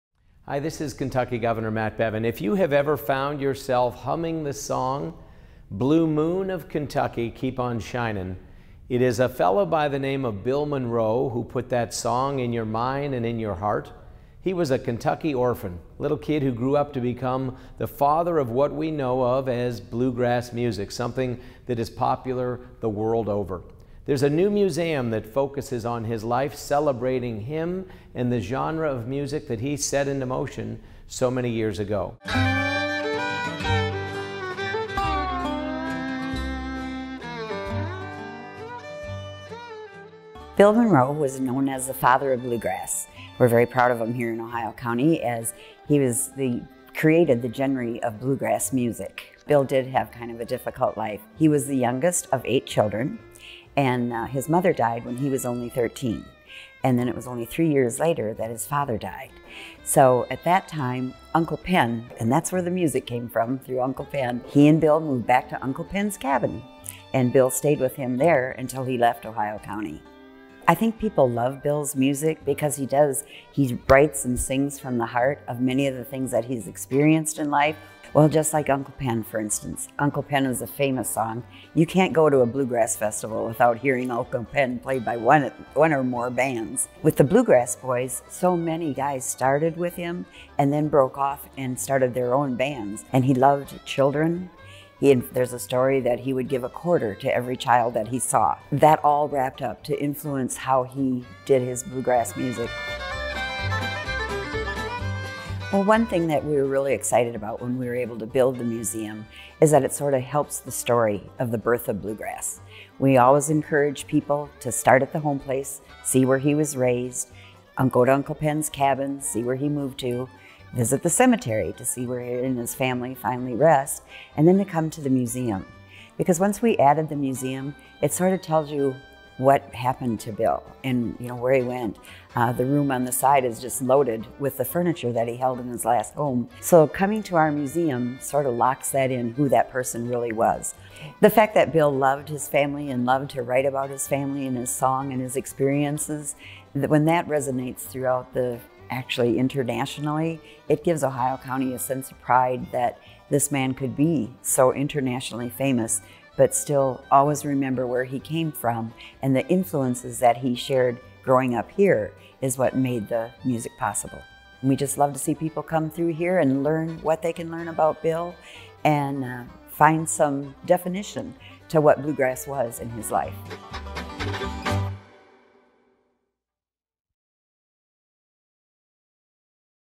Bill Monroe Museum
Gov. Bevin highlights the Bill Monroe Museum in Ohio County, Kentucky.